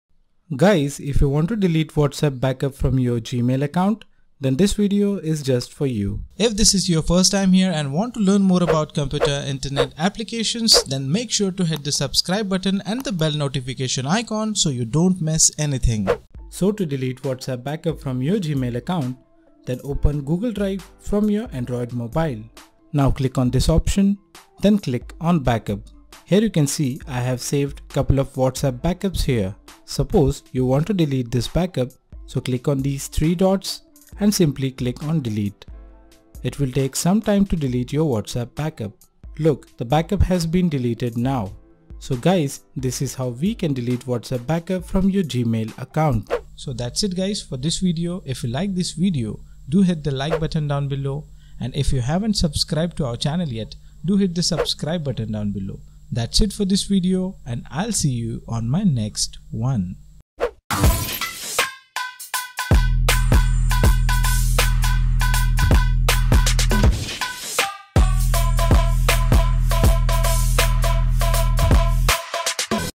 How to Delete Whatsapp Backup from gmail account
This video will show you how you can delete WhatsApp backup from your Gmail account.